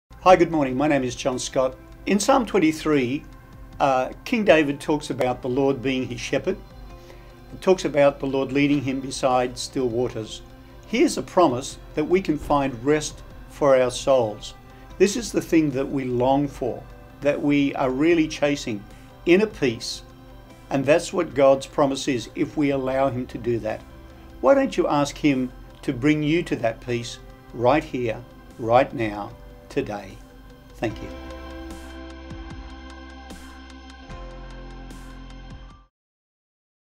Sunday Word of Encouragement: John Scott Psalm 23:1-2
Do you long for rest? God promises rest for our souls if was ask him.